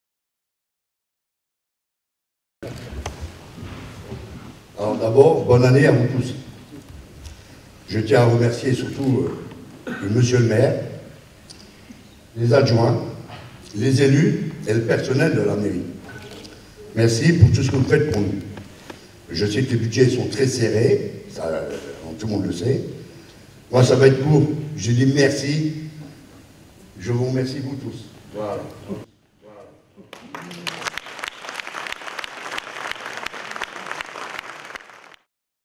2018 KNUTANGE VOEUX ASS BONNE BOULE
Voeux municipaux 2018 de Knutange
Lionel Barbier Président de l'Ass."la bonne boule"